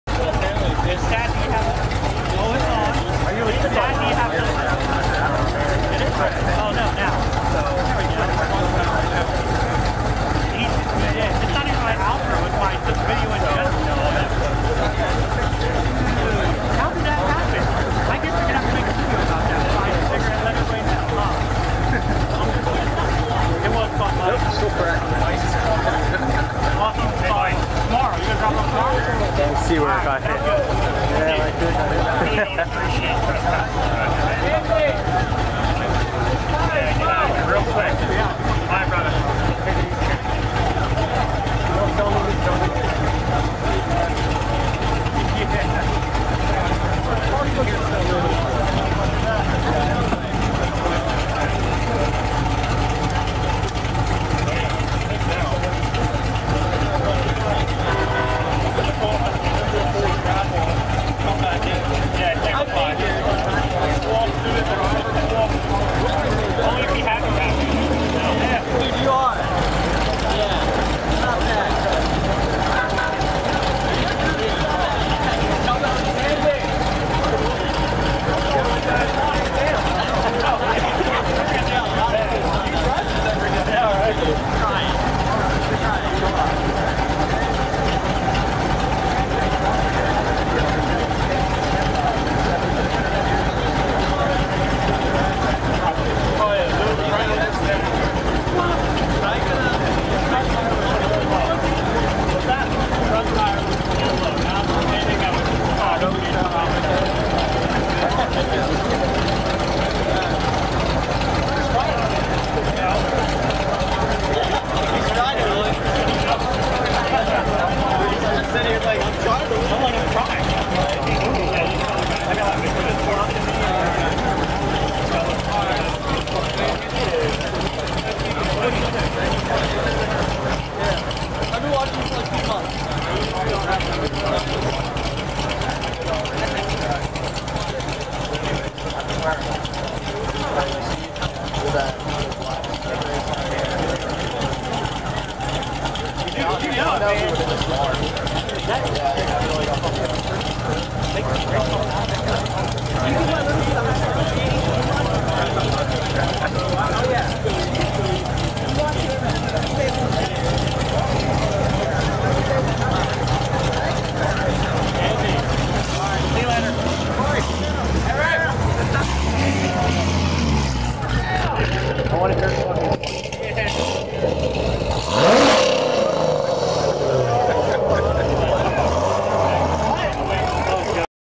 Went To Cars and Coffee Lehigh Valley
I got a clip of Street Speed 717's Z06 leaving Cars and Coffee.
Sorry for the bad angle...